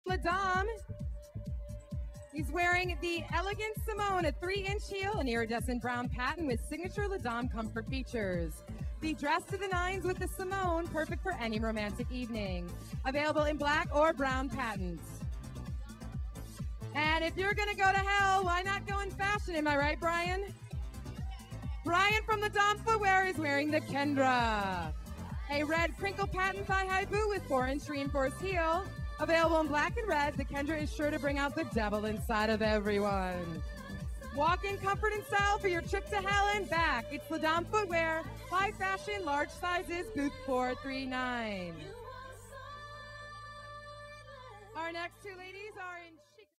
Le Dame 2010 International Lingerie Show part 2
Kiki and Brian walk the runway at the 2010 International Lingerie Show sporting Le Dame Footwear.